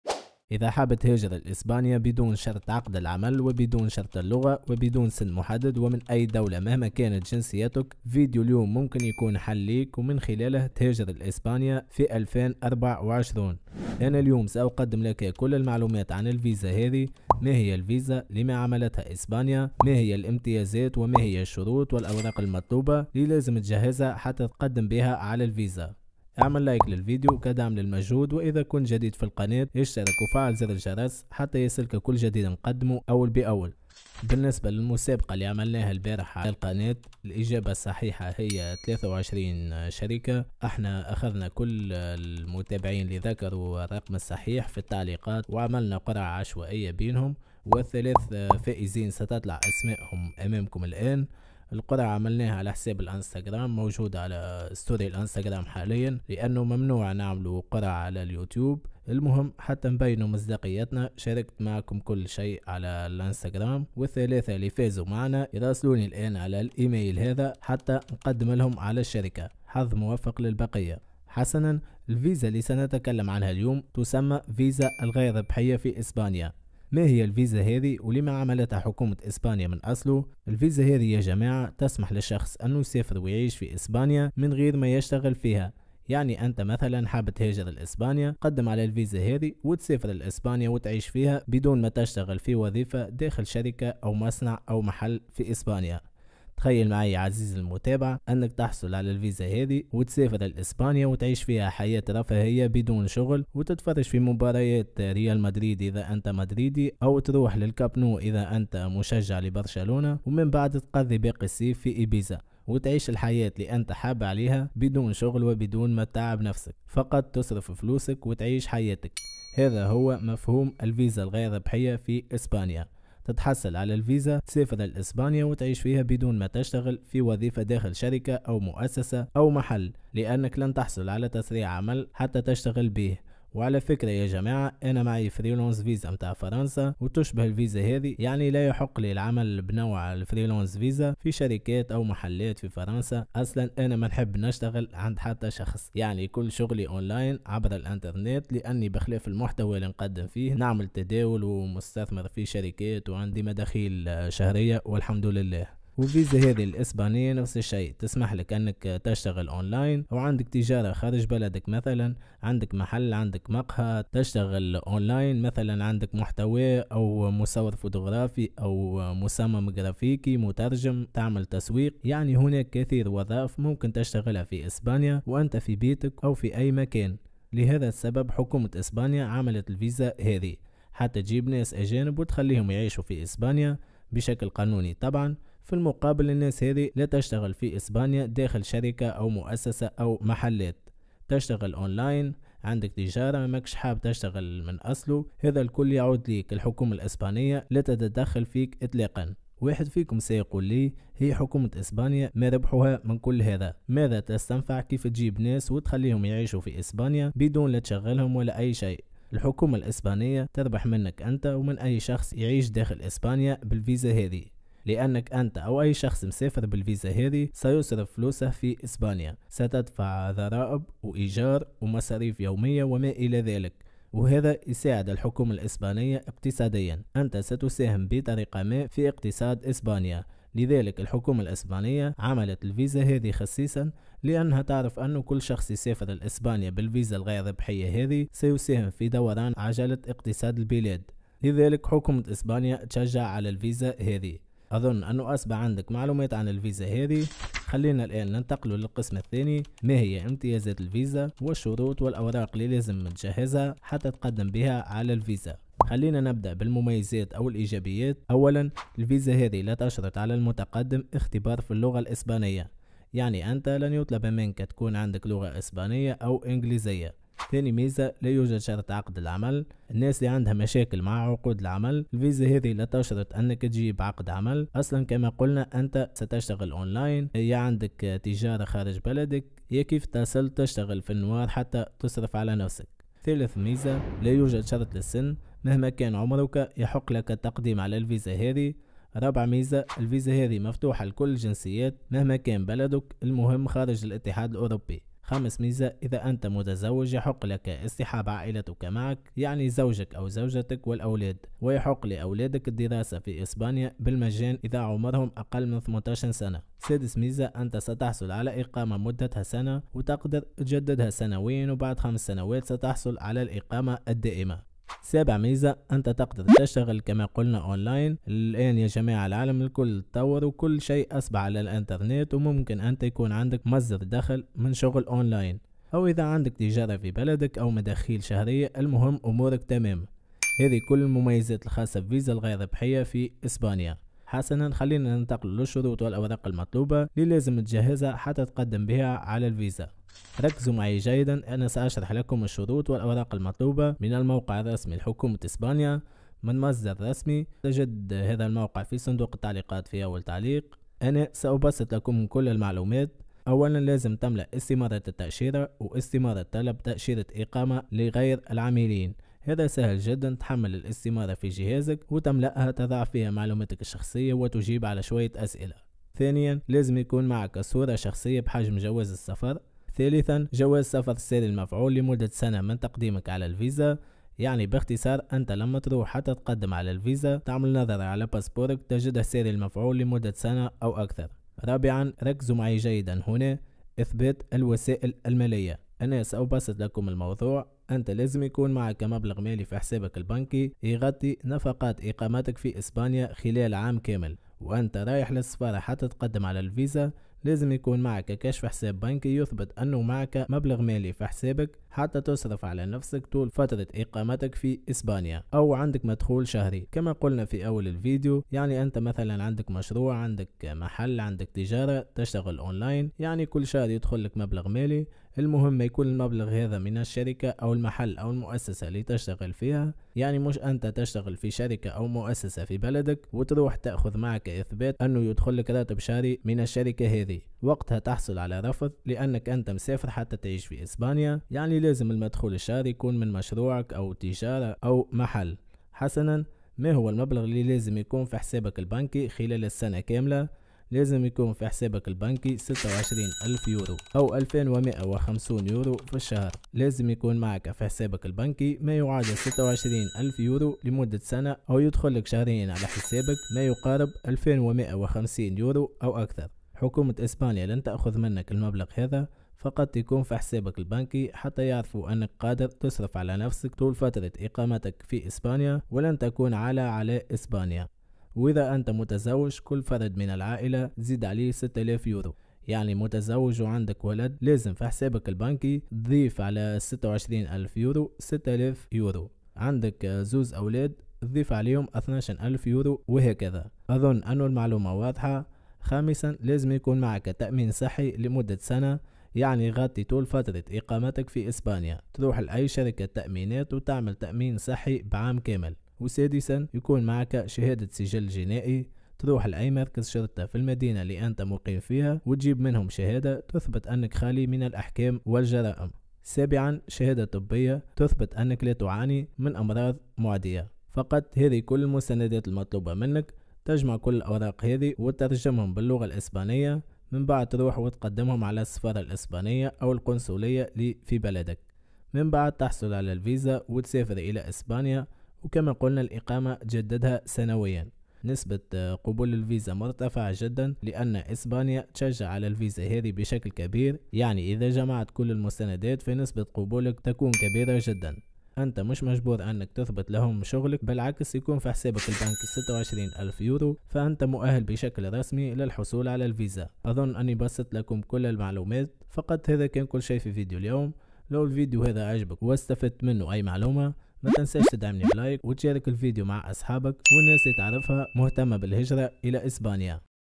فيزا جديدة للهجرة الى إسبانيا بنسبة قبول 97% | بدون عقد العمل و بدون شرط السن و اللغة |لكل الجنسيات
سنعرض لكم في فيديو اليوم
فيزا جديدة للهجرة الى إسبانيا في 2024
الفيزا هذه سهلة جداً و بدون عقد العمل و اللغة و أيضاً بدون شرط السن
الفيزا متاحة لكل الجنسيات و نسبة القبول عالية جدا
ستجد في الفيديو كل المعلومات الخاصة بالفيزا و الامتيازات و الاوراق المطلوبة